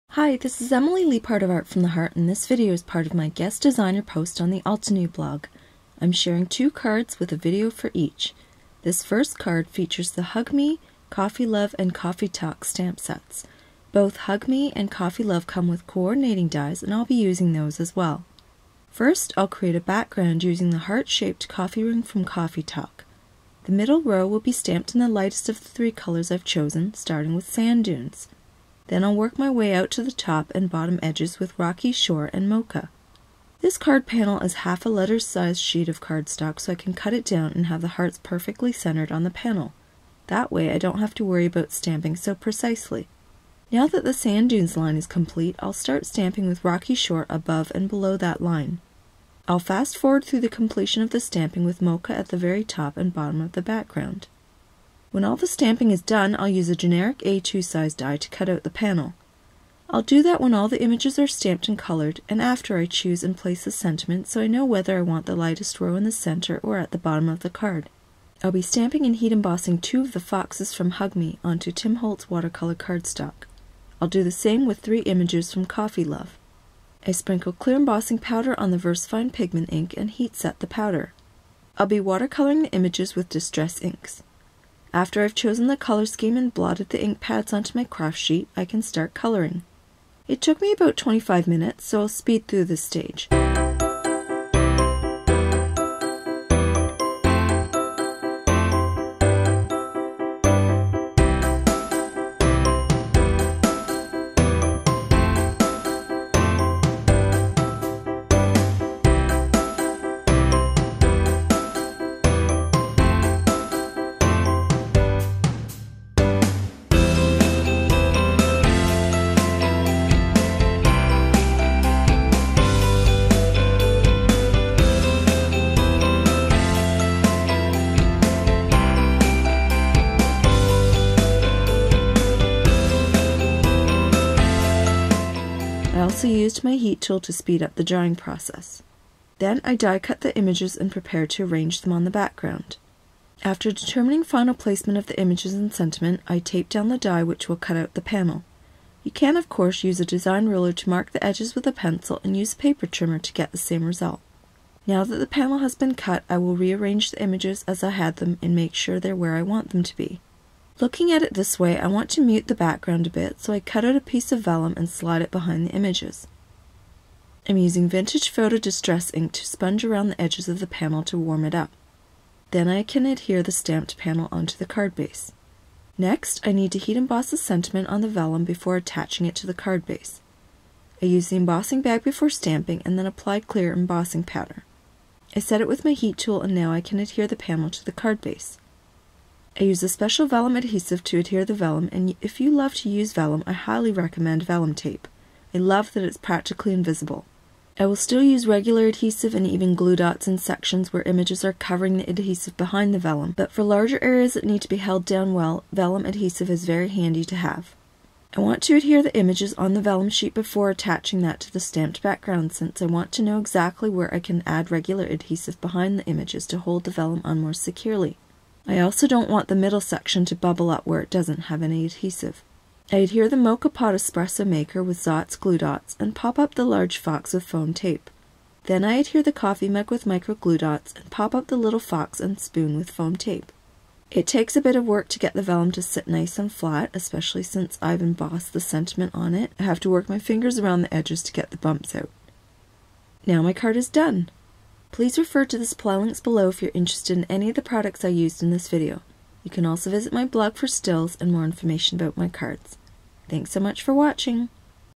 Perk Up - Watercoloring with Distress Inks
Altenew guest designer Emily Leiphart shares how she created a handmade card featuring two adorable foxes from Hug Me, combined with images from Coffee Love and a sentiment and background from Coffee Talk stamp set. More details on the blog